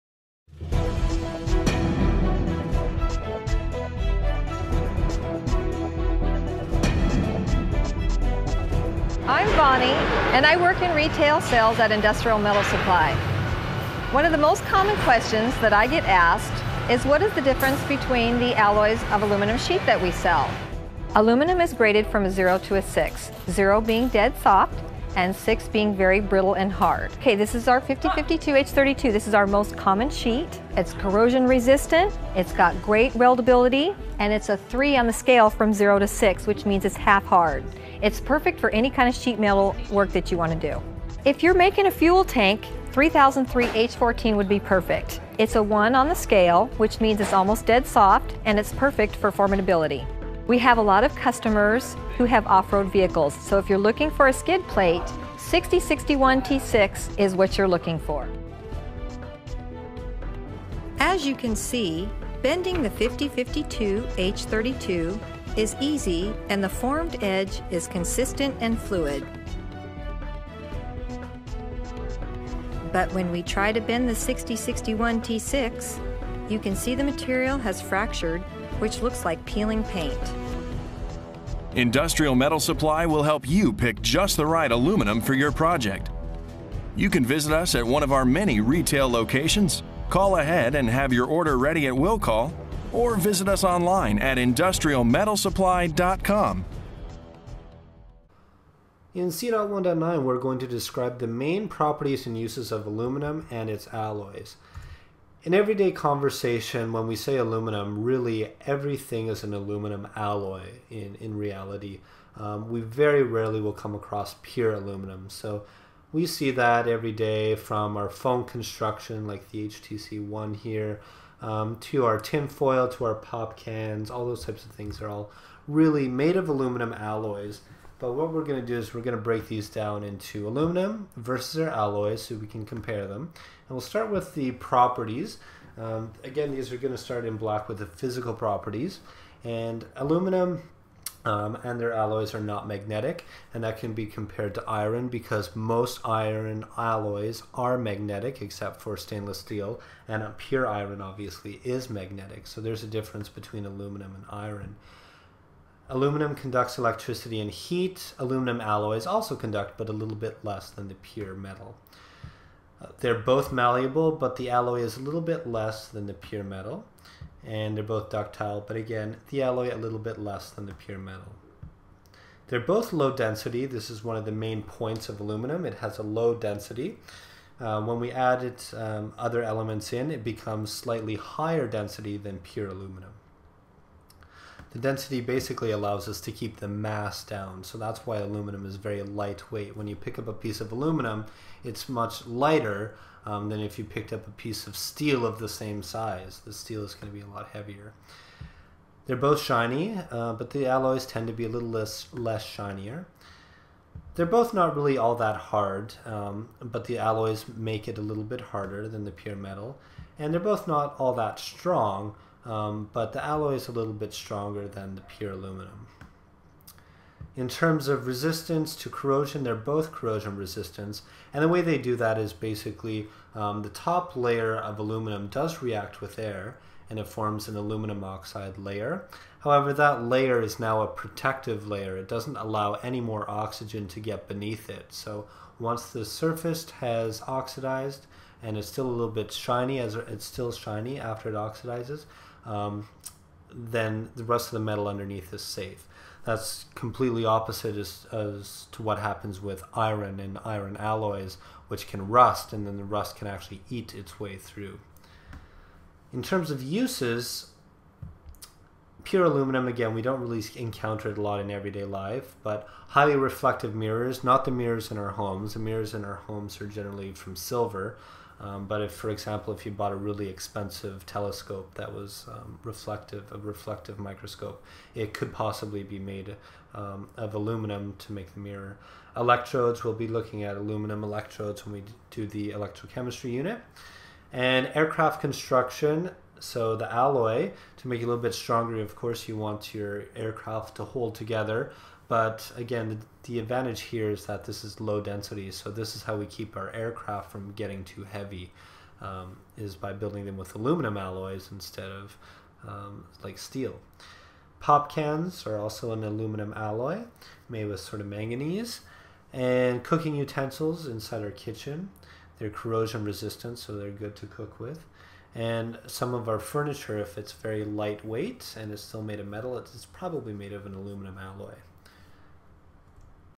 C.1.9 - Describe the main properties and uses of aluminum and its alloys.
This is part of the Sustainability Through Chemistry approach to IB Chemistry, currently under development.